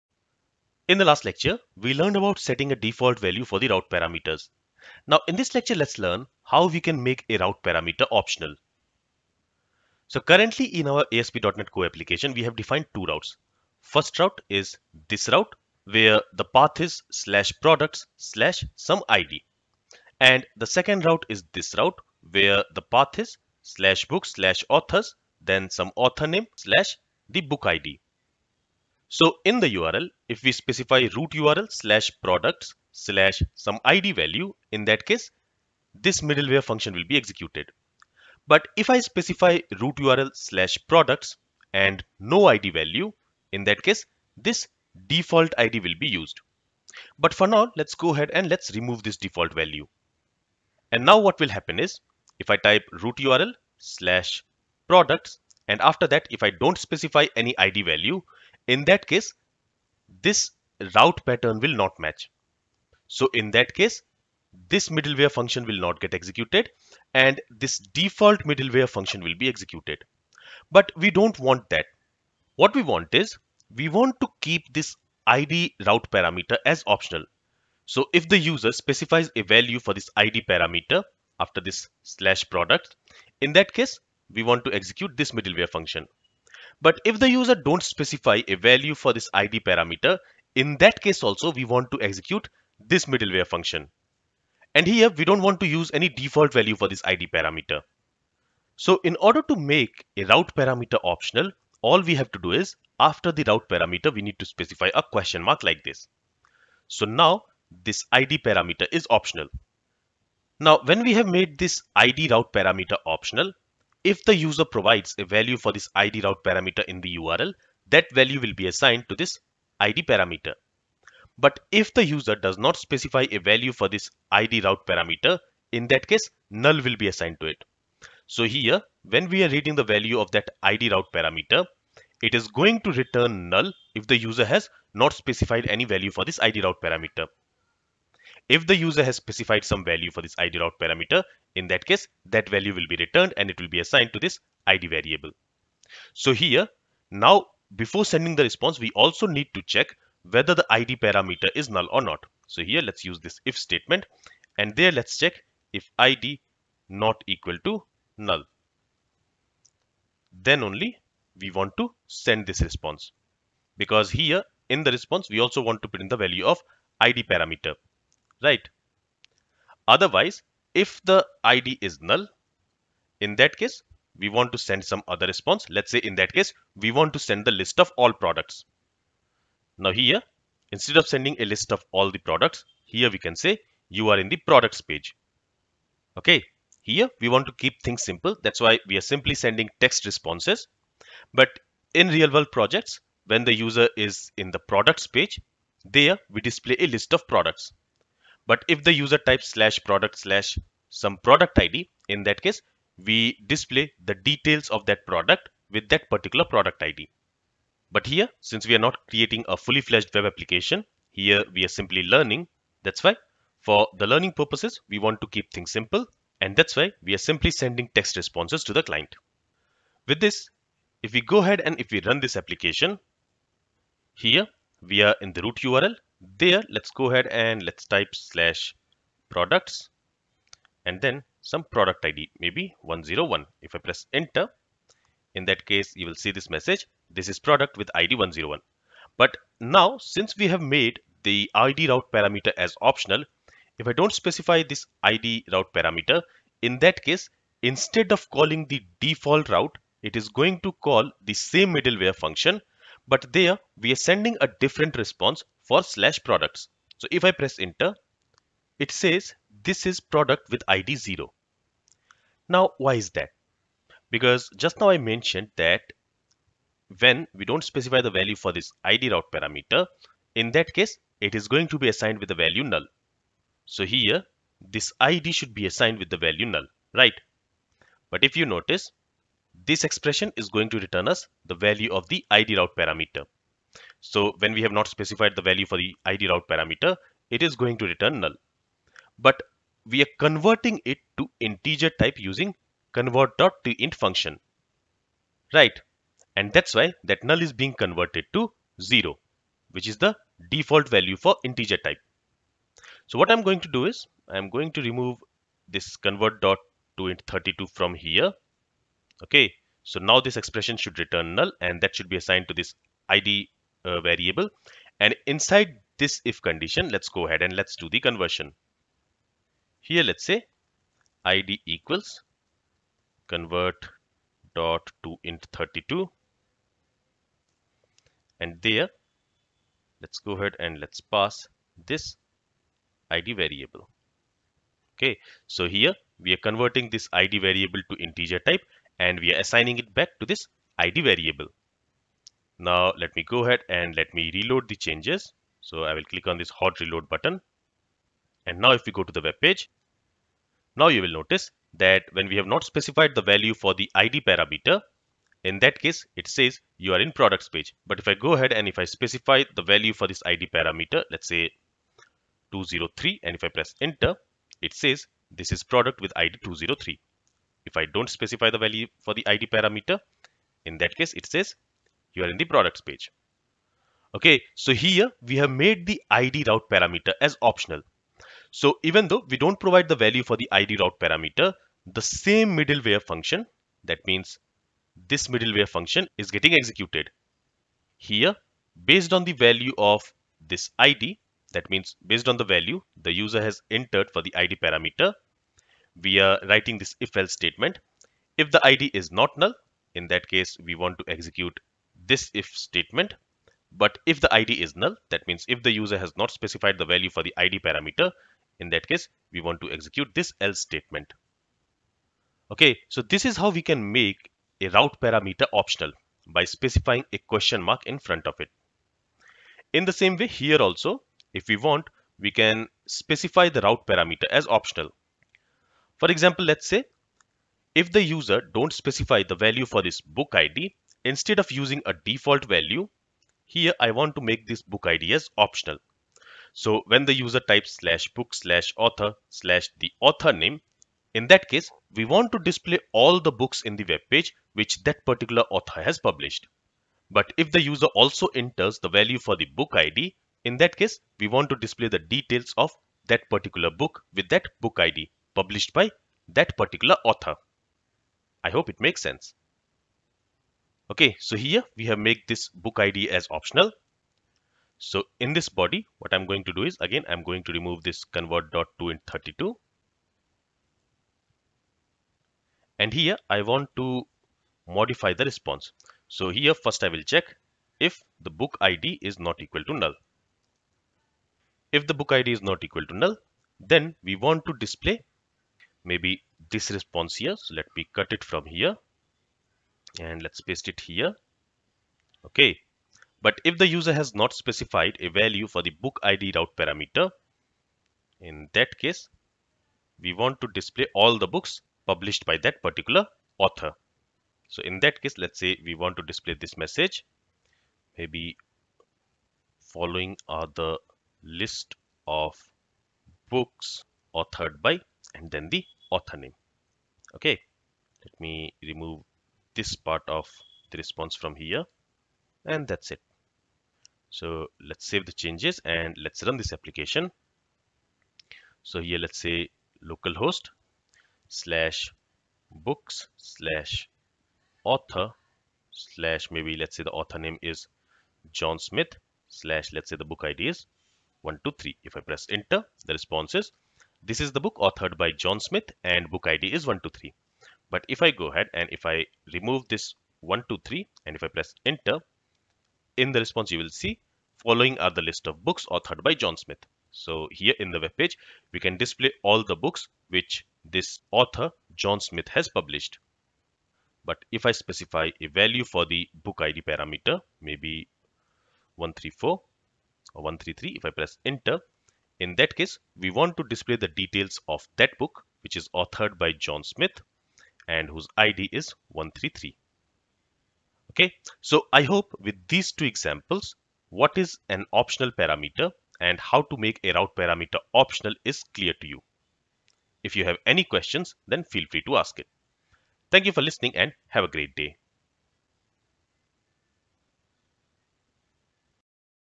#28 Optional Route Parameter | Understanding Routing | ASP.NET Core MVC Course
In this lecture let's learn how to set a route parameter as optional. Currently, if we do not specify the route parameter value in the URL, either we get an error or the default route will be executed.

But when we make the route parameter as optional, in that case even if we do not specify the value of route parameter, the end point for that route will be executed. Let's understand this with example.